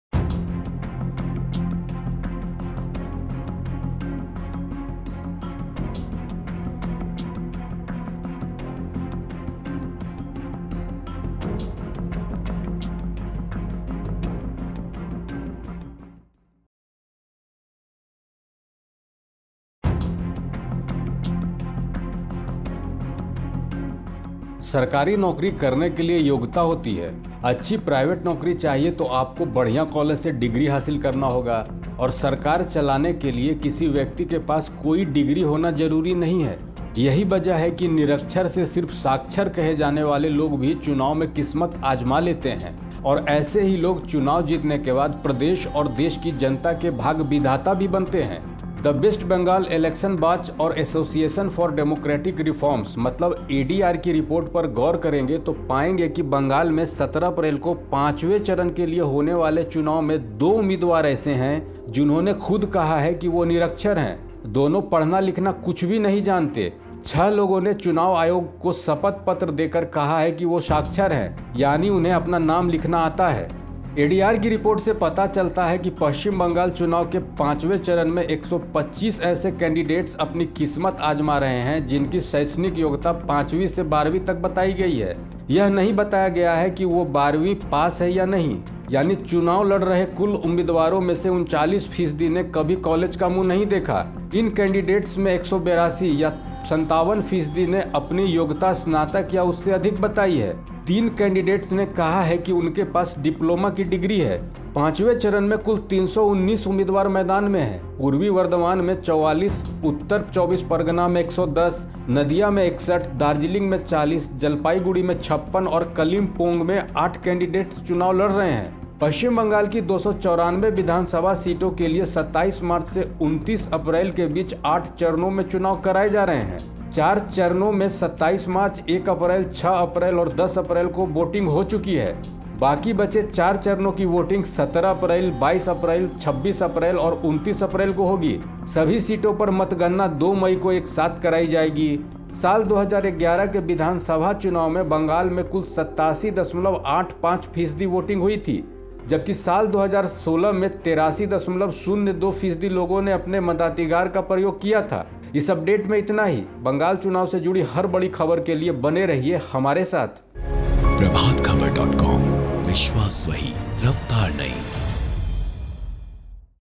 Bengal Election 2021: Fifth Phase में 125 Candidates ने नहीं देखा है कॉलेज का मुंह | Prabhat Khabar
सरकारी नौकरी करने के लिए योग्यता तय होती है. अच्छी प्राइवेट नौकरी चाहिए तो आपको बढ़िया कॉलेज से डिग्री हासिल करना होगा. सरकार चलाने के लिए किसी व्यक्ति के पास कोई डिग्री होनी जरूरी नहीं है. यही वजह है कि निरक्षर से सिर्फ साक्षर कहे जाने वाले लोग भी चुनाव में किस्मत आजमा लेते हैं. ऐसे ही लोग चुनाव जीतने के बाद प्रदेश और देश की जनता के भाग्य विधाता बन जाते हैं. द वेस्ट बंगाल इलेक्शन वाच और एसोसिएशन फॉर डेमोक्रेटिक रिफॉर्म्स मतलब एडीआर की रिपोर्ट पर गौर करेंगे, तो पायेंगे कि बंगाल में 17 अप्रैल को पांचवें चरण के लिए होने वाले चुनाव में दो उम्मीदवार ऐसे हैं, जिन्होंने खुद कहा है कि वो निरक्षर हैं. दोनों पढ़ना-लिखना कुछ भी नहीं जानते. छह लोगों ने चुनाव आयोग को शपथ पत्र देकर कहा है कि वो साक्षर हैं. यानी उन्हें अपना नाम लिखना आता है.